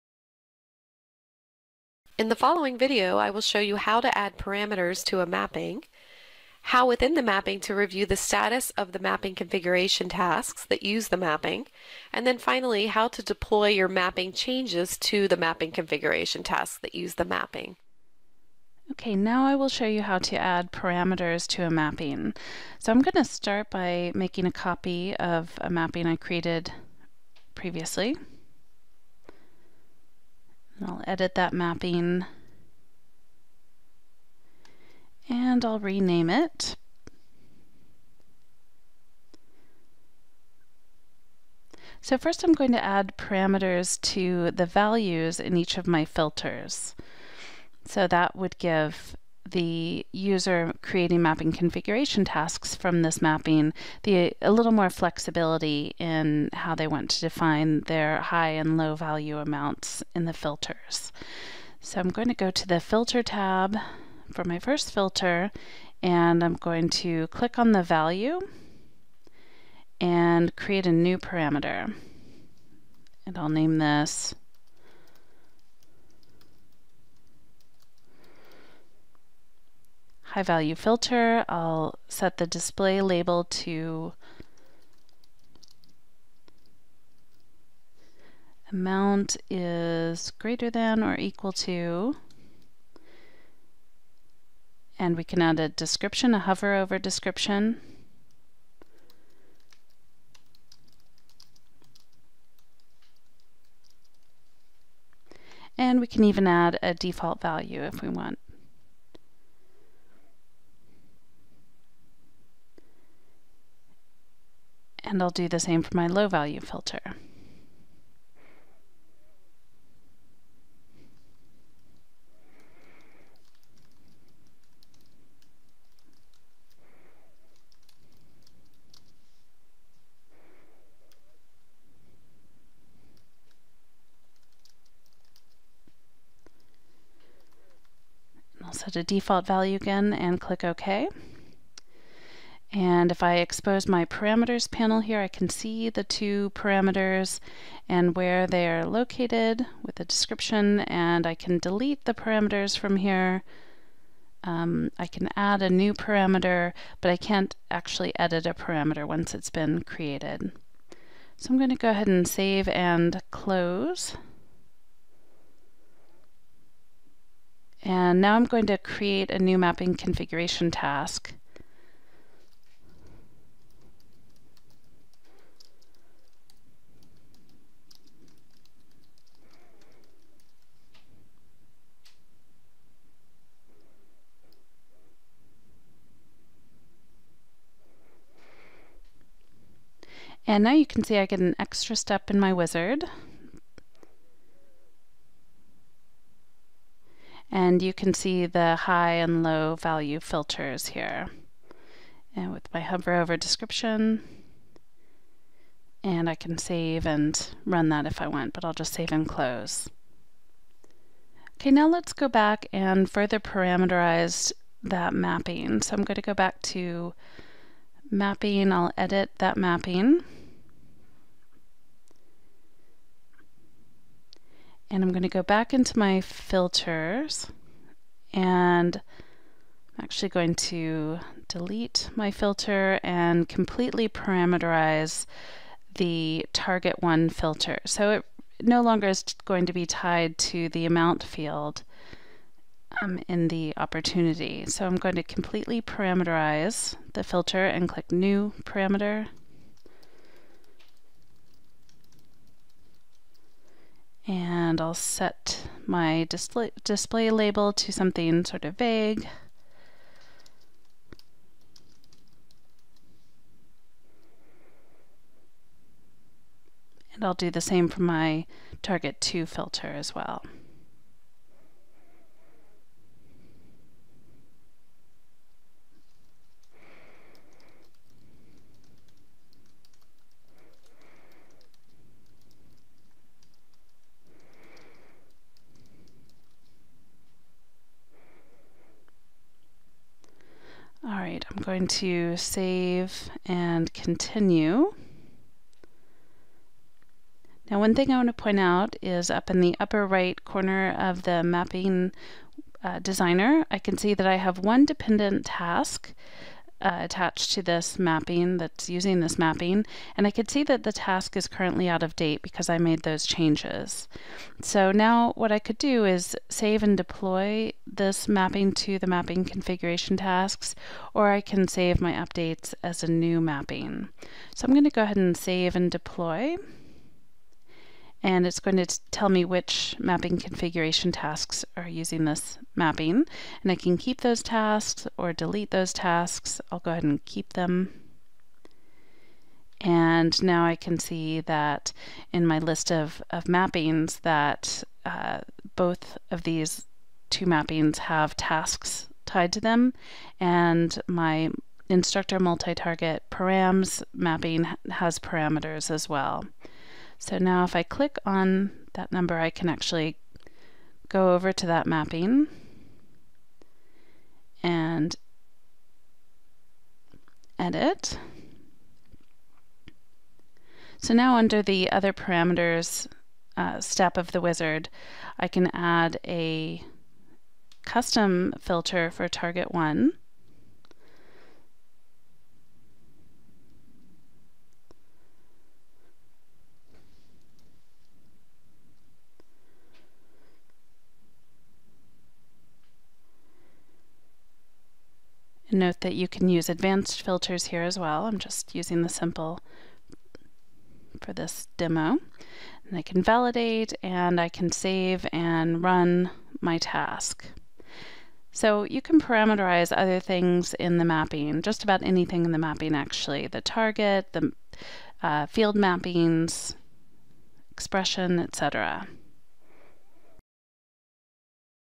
Informatica Cloud: Parameterized Mappings
This video demonstrates how to add parameters to a mapping.